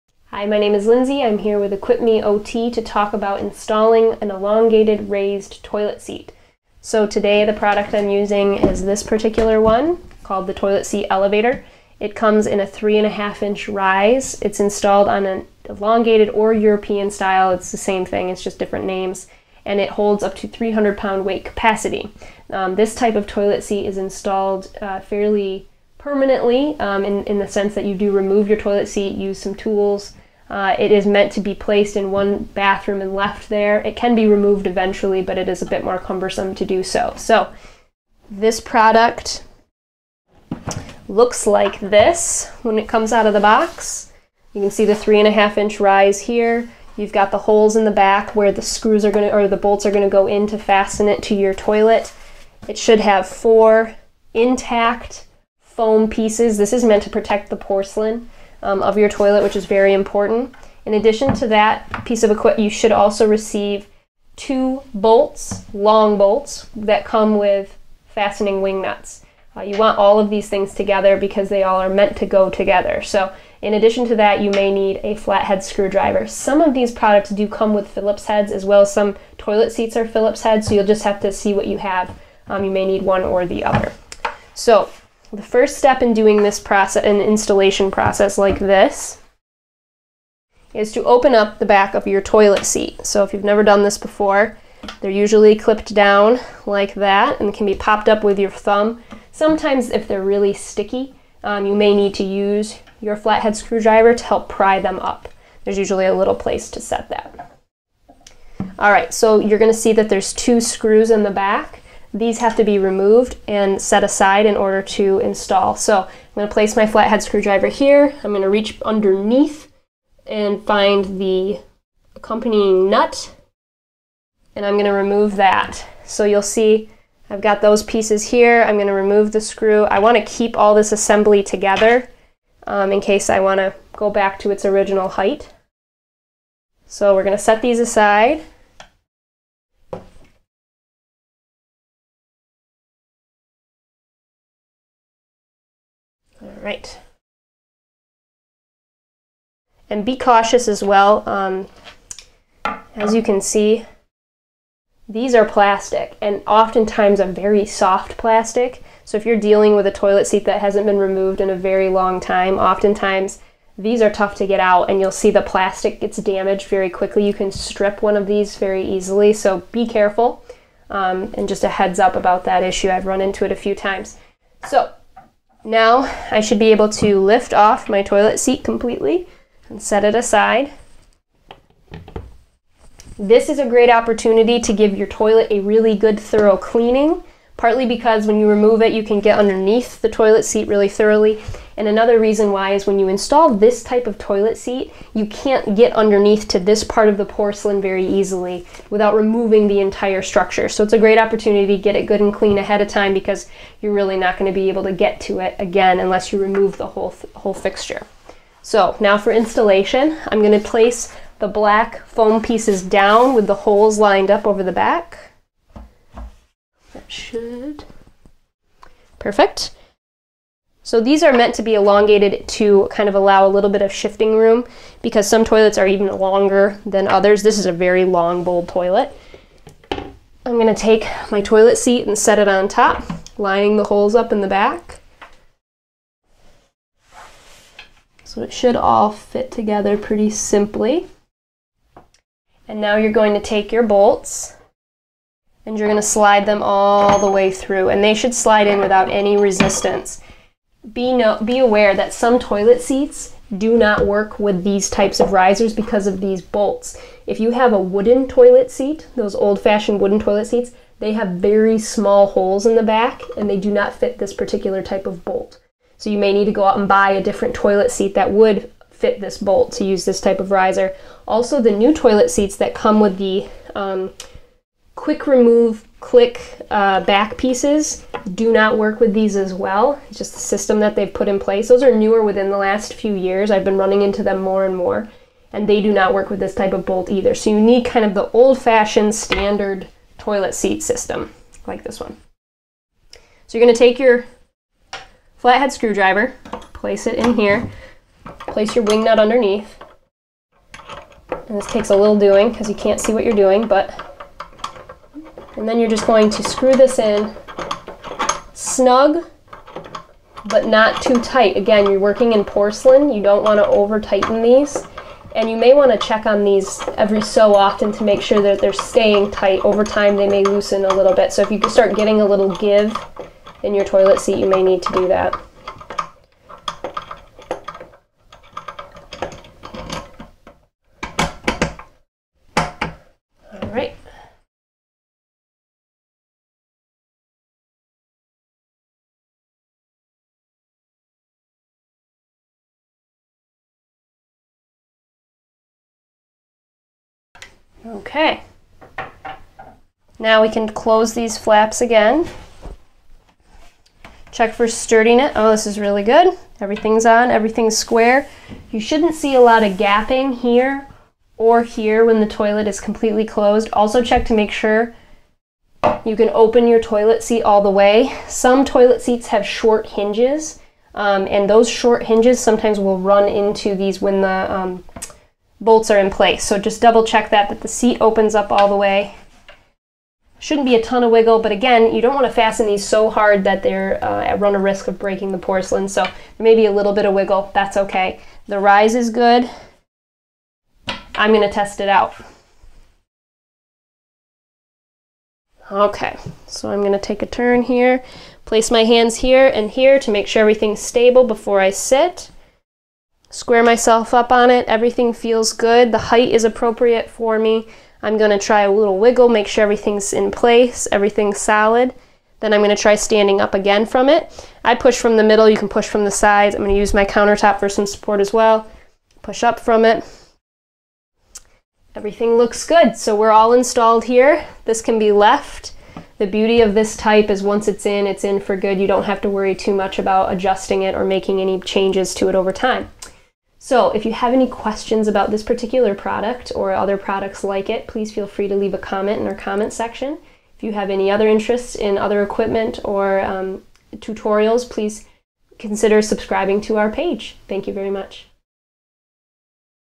How to Install a Raised Toilet Seat with Assembly | Carex Raised Toilet Seat
Step by step video tutorial on how to install a raised toilet seat that mounts under the toilets existing seat.

Product Link:
Carex Raised Toilet Seat